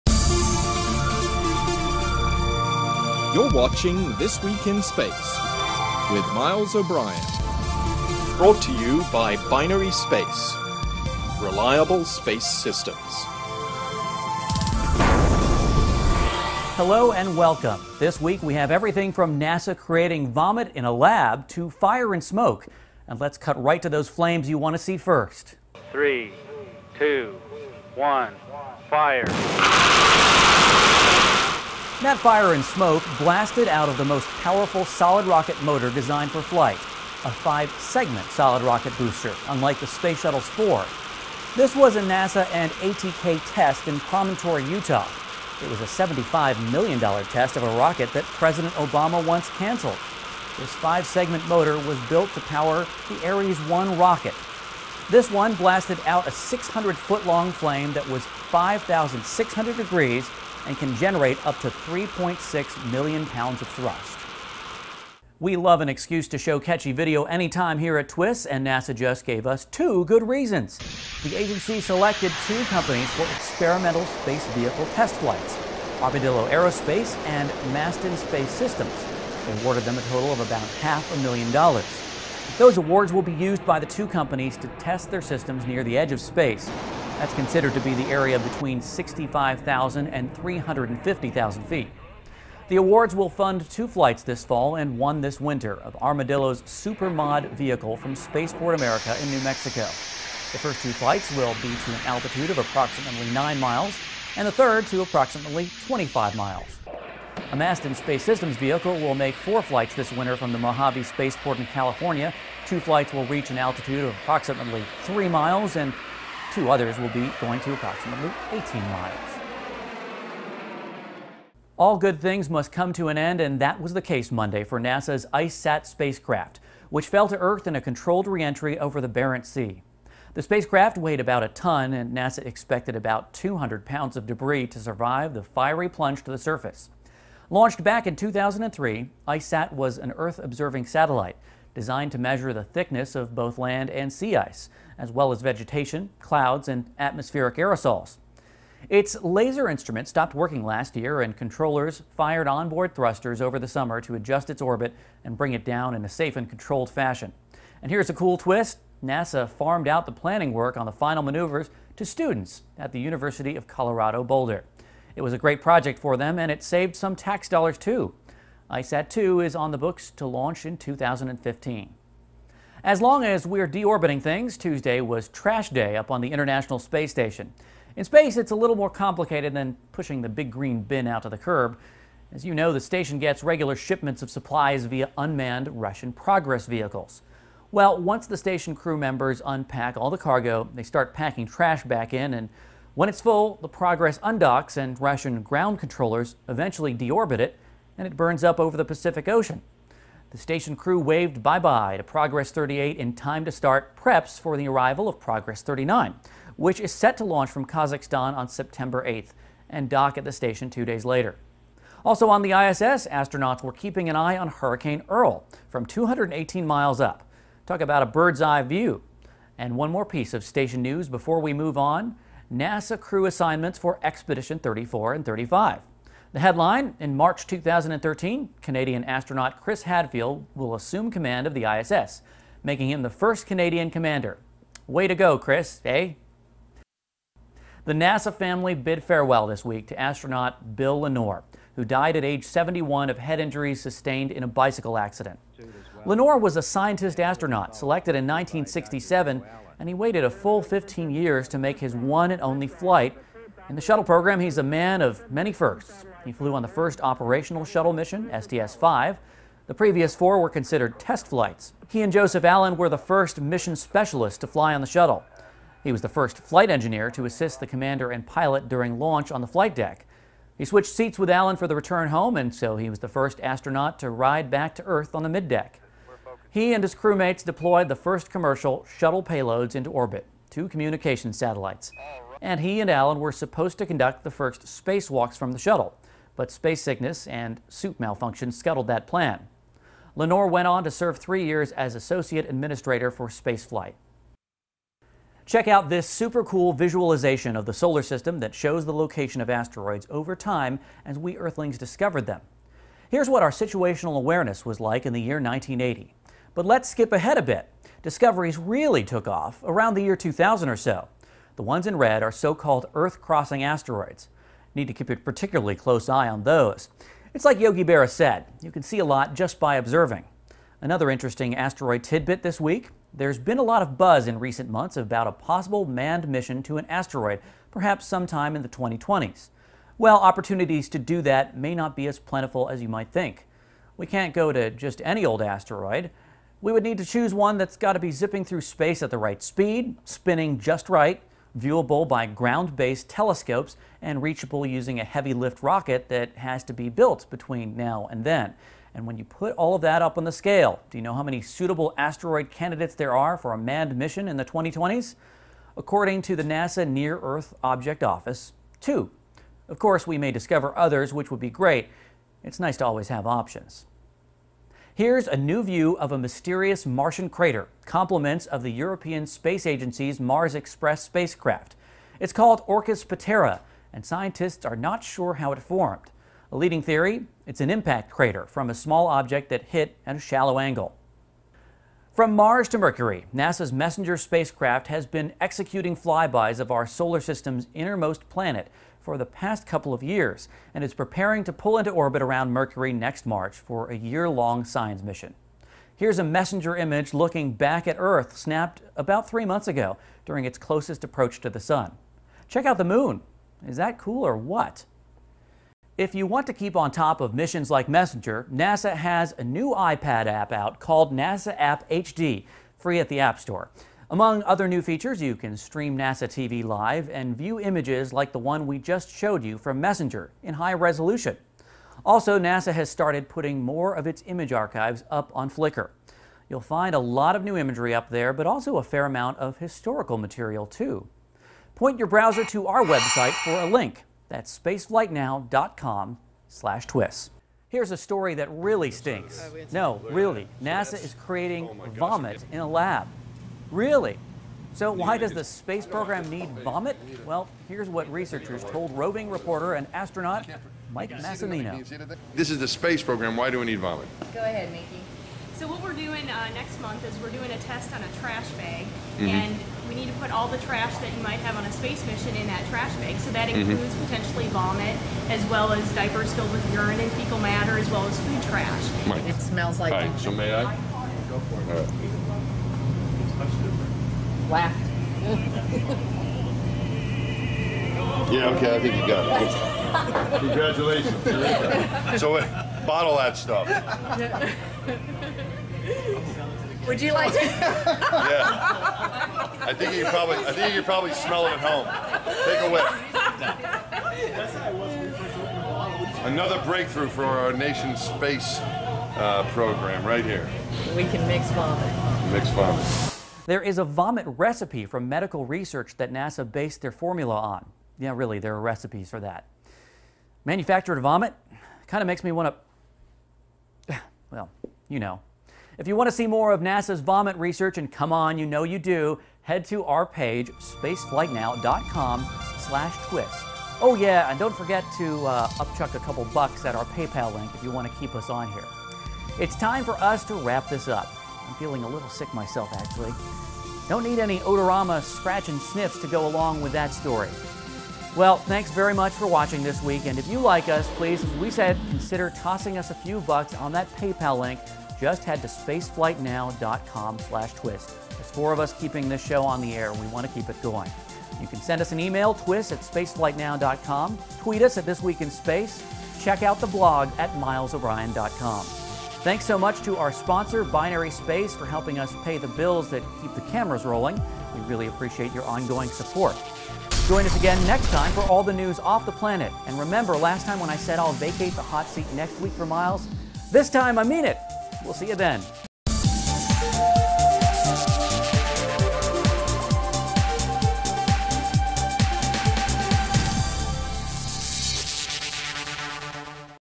This Week in Space - September 5, 2010
A massive booster roars to life in Utah test, taking out the trash space station style, planning an asteroid mission and NASA mixes up fake vomit in the lab!